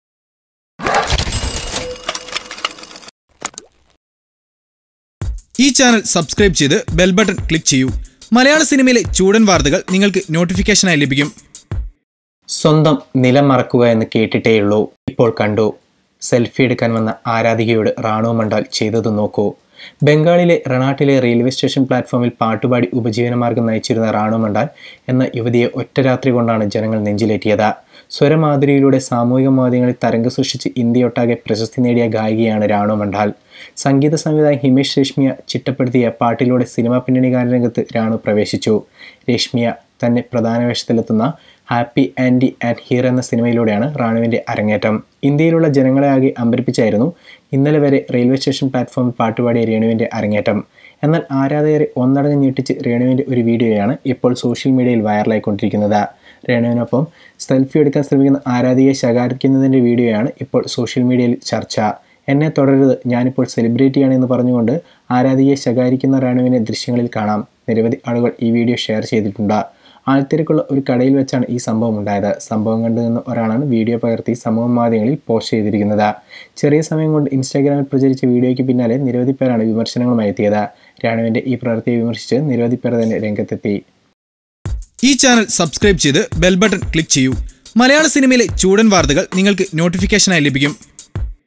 ഗയികയായപ്പോള്‍ എല്ലാം മറന്നു ഫോട്ടോ എടുക്കാൻ വന്ന ആരാധികയോട് രാണു മൊണ്ഡാല്‍ ചെയ്തത്
Watch ഒരു പാട്ടുപാടി വലിയ ആളായപ്പോഴേക്കും എല്ലാം മറന്നു ഫോട്ടോ എടുക്കാൻ വന്ന ആരാധികയോട് രാണു മൊണ്ഡാല്‍ ചെയ്തത് നോക്കൂ